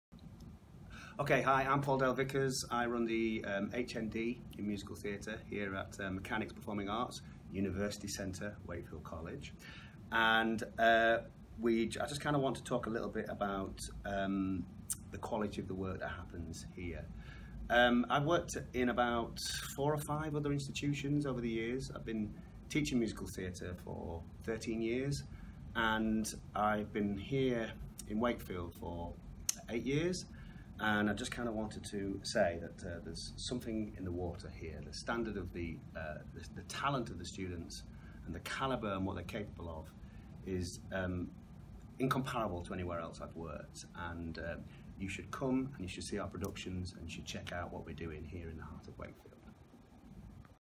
Performing Arts Summer Festival at Wakefield College with Paul Dale
Paul Dale introducing this year's Performing Arts Summer Festival!